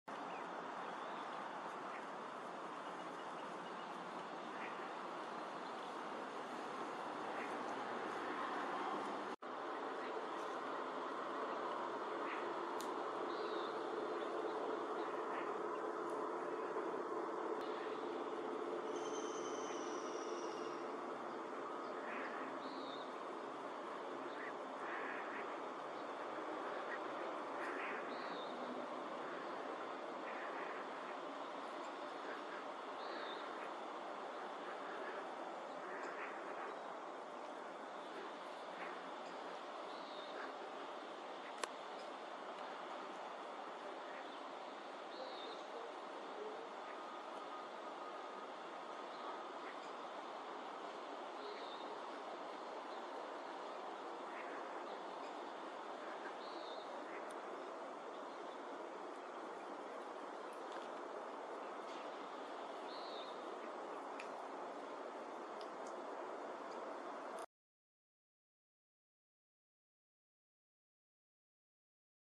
Wild Turkey hen with chicks
On Jackson Street, Quincy, CA, in front of my apartment.  August 14, 2023